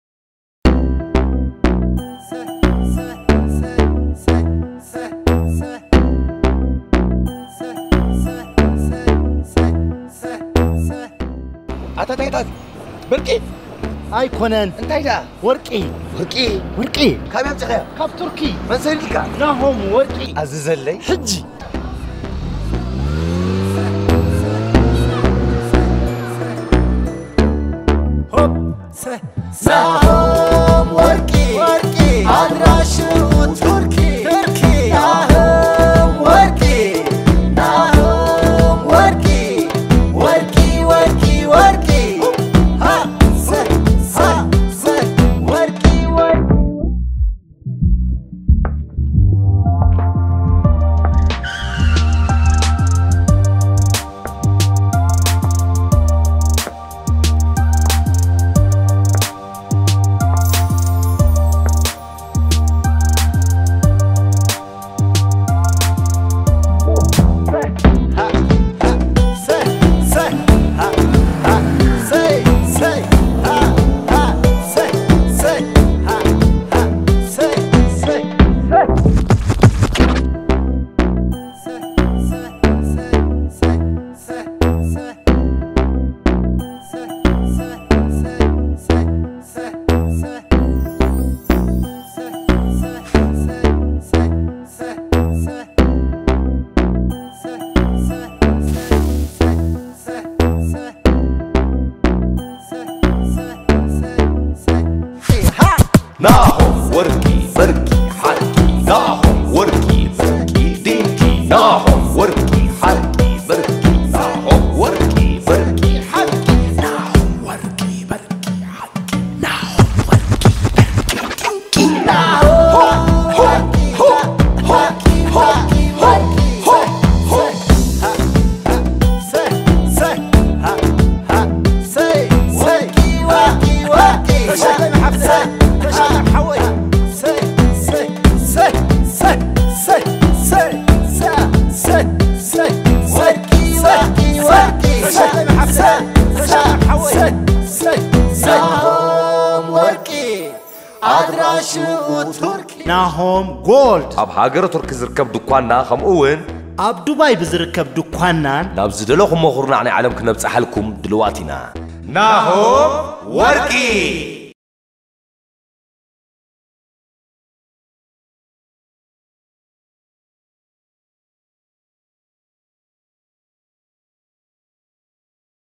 Yonas Maynas - Nahom Gold - ናሆም ወርቂ ኣድራሽኡ ቱርኪ - Eritrean Comedy Commercial
Nahom Gold - Eritrean Comedy Commercial by Yonas Maynas and Tenno.